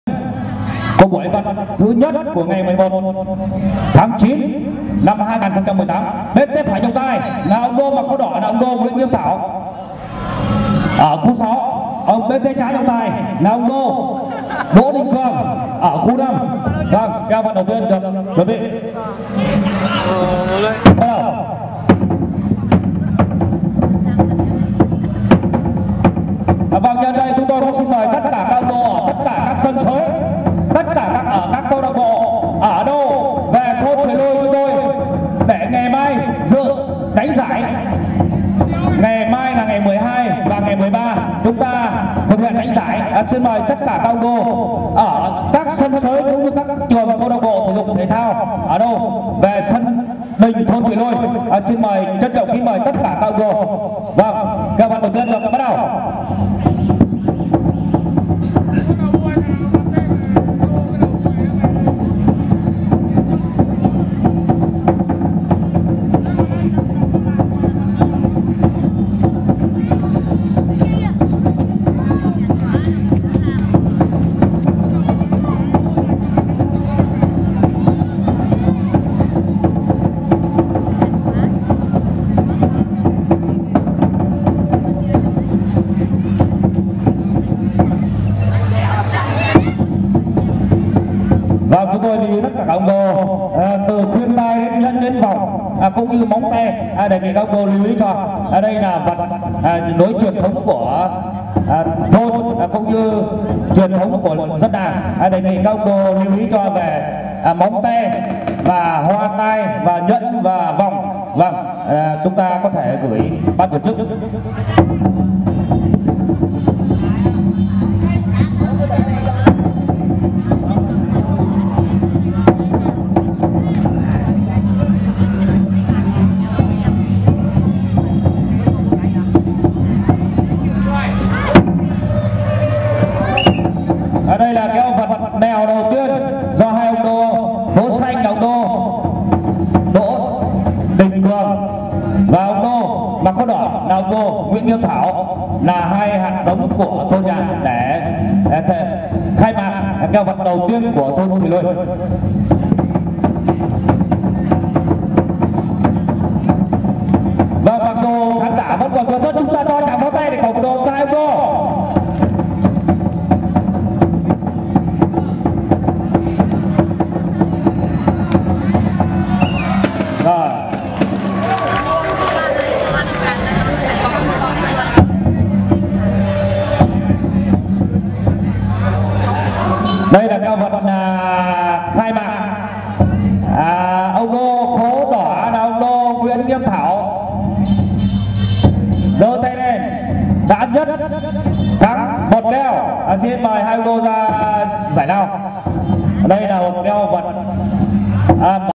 Hội vật Thụy Lôi 2018 Nguyễn Nghiêm Thảo (Thụy Lôi) VS ?
Làng Nhội xưa, nay là thôn Thụy Lôi, xã Thụy Lâm, huyện Đông Anh, Hà Nội.
Hội làng và giải Vật có thể nói là muộn nhất trong năm ở các làng quê miền Bắc.
Đình làng Nhội hiện có kiến trúc rất đẹp. Tương truyền là đình của An Dương Vương ngự khi về bái yết Huyền Thiên Chấn Vũ. Có lẽ vì vậy ngôi đình không thờ thành hoàng làng như các ngôi đình làng Việt Nam khác mà chỉ thờ thần.
Hội làng được tổ chức vào 13-15 tháng 9 âm lịch hàng năm.
Sới vật ở ngay chái Đình.
Video này được trang fanpage Vật Việt Nam Vietnam Wrestling quay trực tiếp tại đây.